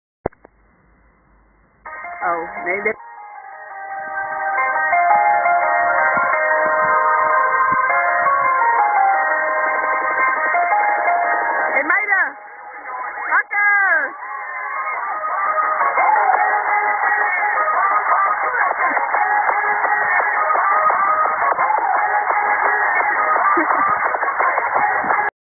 Hakken na vogelvrij
Na een festival wachten om opgehaald te worden, ja dan ontstaan dit soort filmpjes, Hey meiden... HAKKUHHH!!!